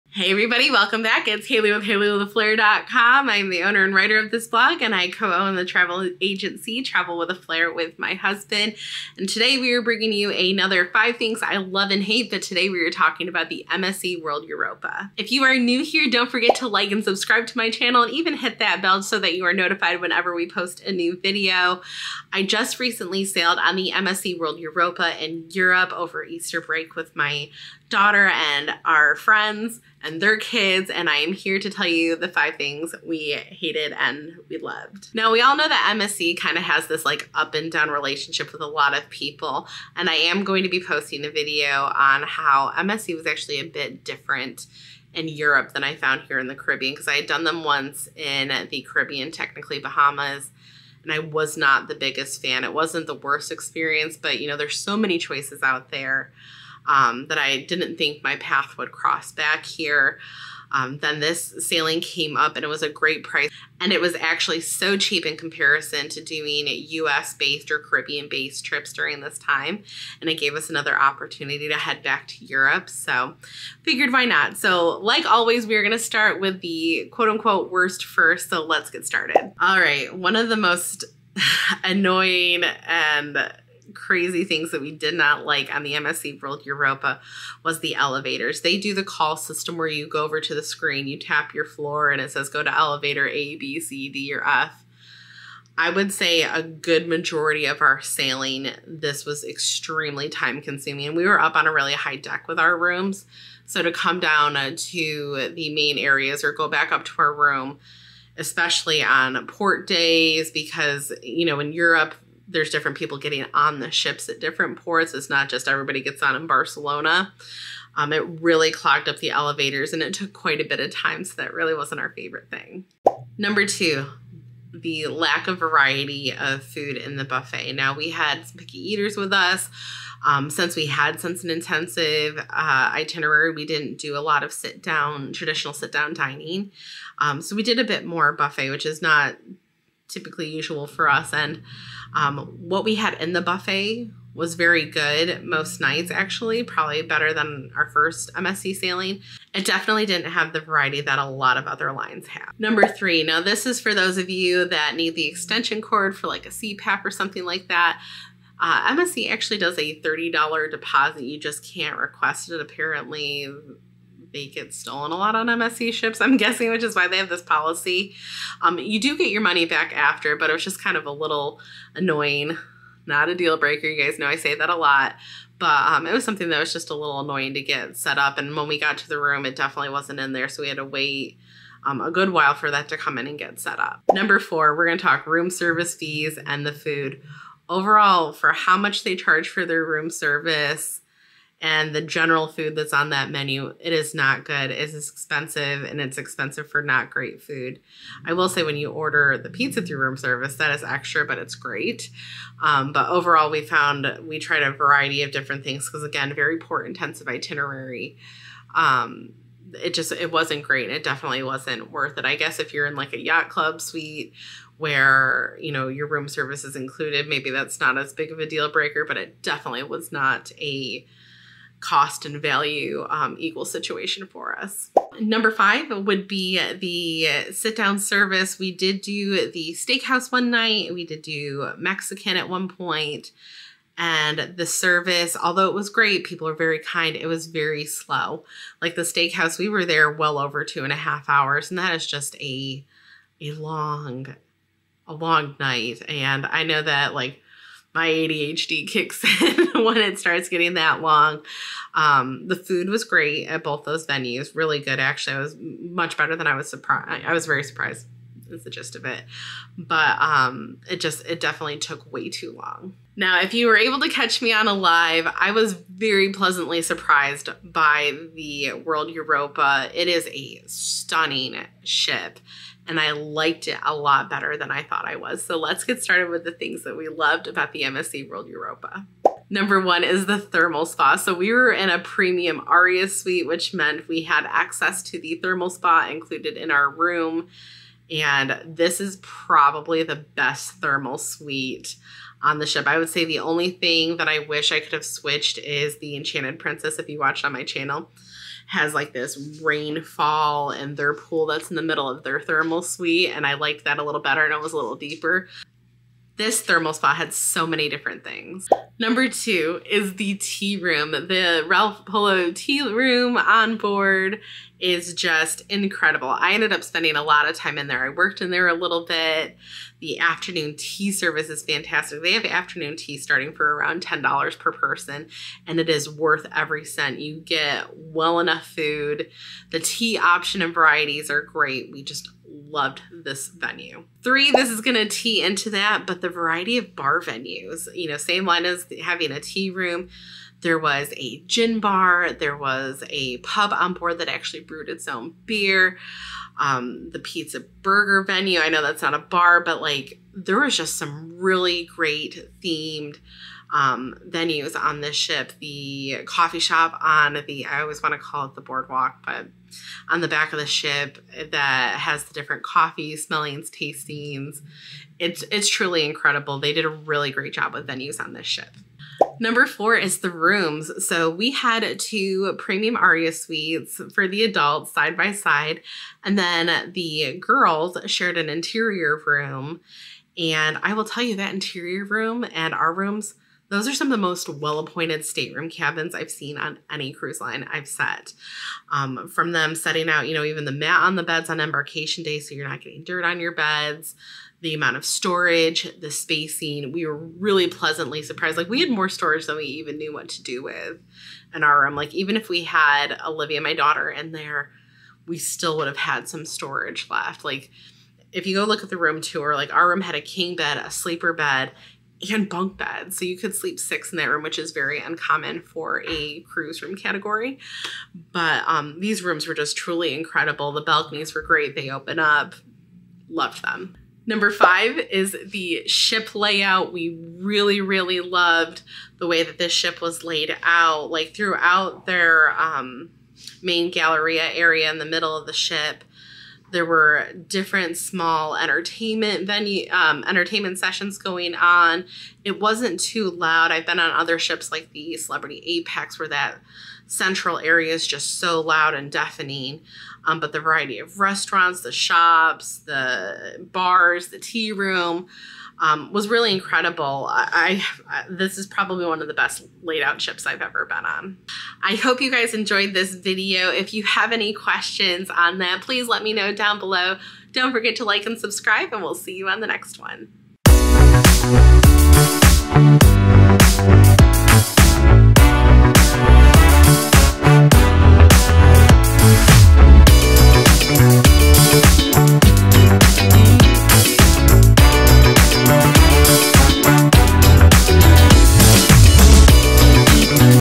5 Things We Loved & Hated about the MSC World Europa | MSC Cruises Europe Sailing | Ship Review
5 Things We Loved & Hated about the MSC World Europa | MSC Cruises Europe Sailing | Ship Review

Air Tag/Medallion Holders

Welcome to our detailed review of the MSC World Europa! In this video, we share our top 5 likes and dislikes about this stunning cruise ship. Whether you're planning a European sailing adventure or just curious about the MSC World Europa, this video is for you. Join us as we dive into the details of our MSC World Europa experience, offering you an honest look at both the highlights and the drawbacks. URL.

TRAVEL GEAR